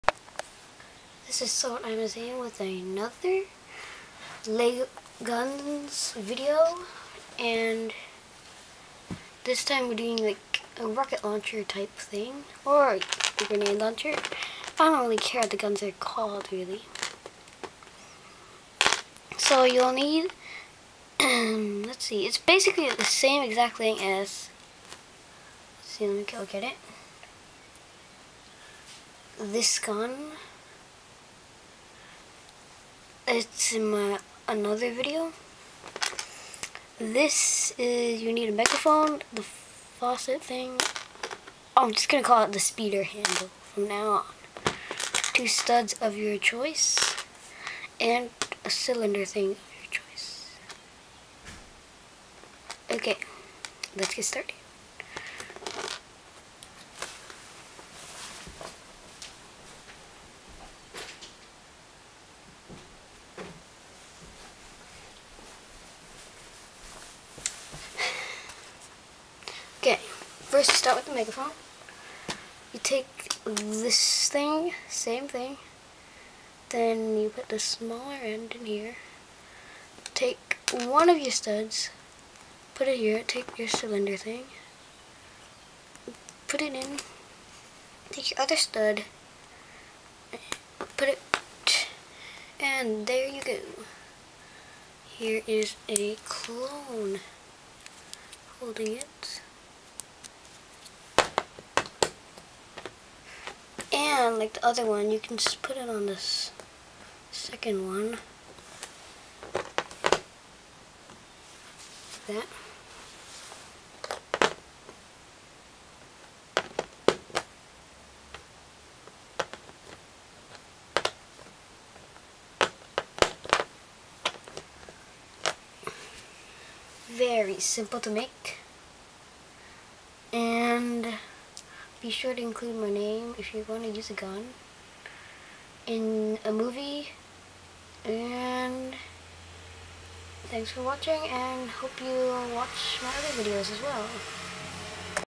Lego HOW TO:Weapons_Rocket Launcher/Grenade Launcher
This is my version on a rocket launcher/grenade launcher.Please rate,comment and subscibe.